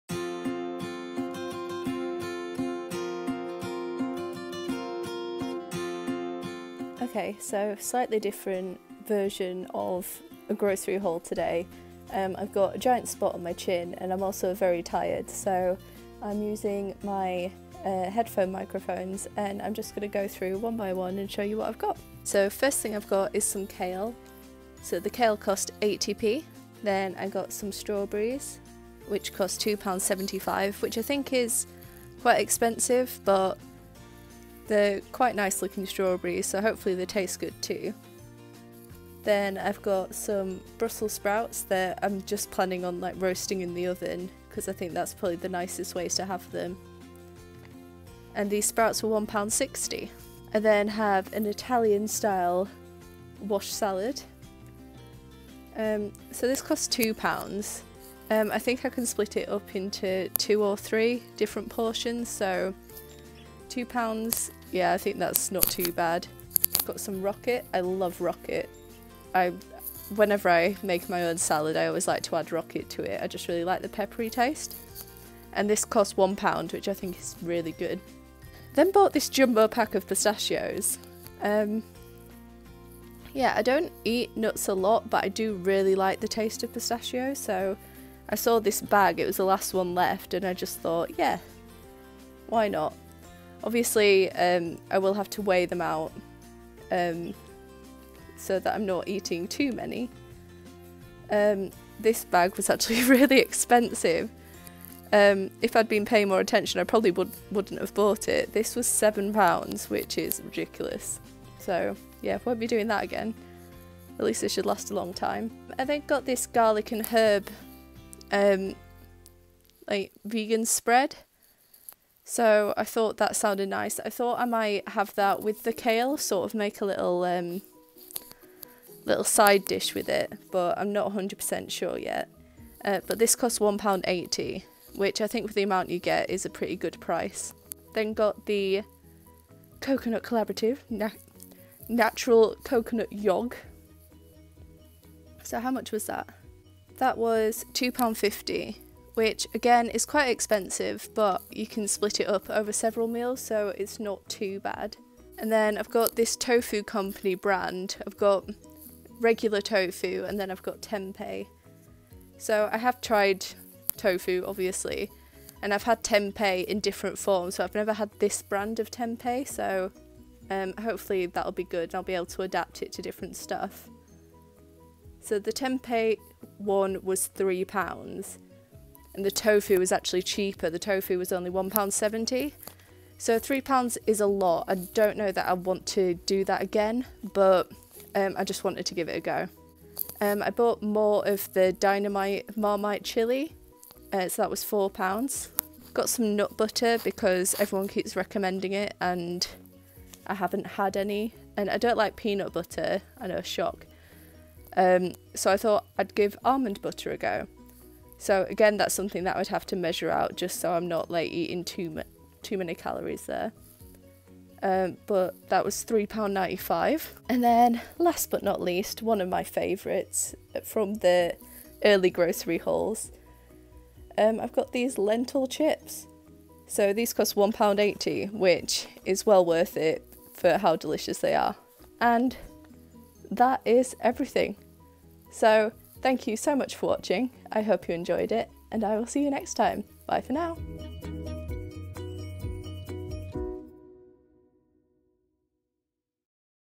Yet Another Vegan Grocery Haul 🌱
Sorry that the mic is crackling at times. Hopefully the video is still interesting and enjoyable for you :D
Also this was filmed before Monday’s update, hence why there’s nuts and nut butter in the haul. This was filmed on Thursday 18th and the update was filmed on Sunday 21st.